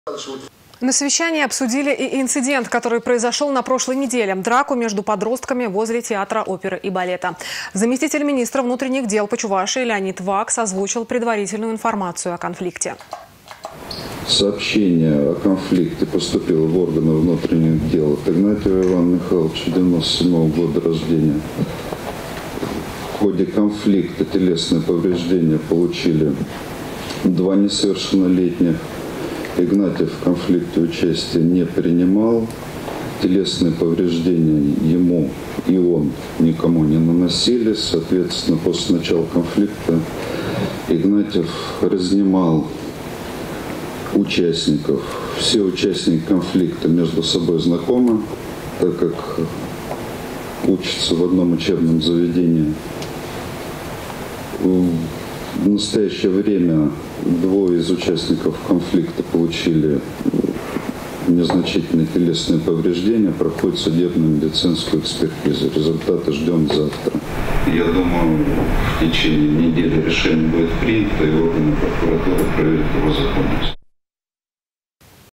На совещании обсудили драку между подростками возле Театра оперы и балета.
Заместитель министра внутренних дел по Чувашии Леонид Вакс озвучил предварительную информацию о конфликте.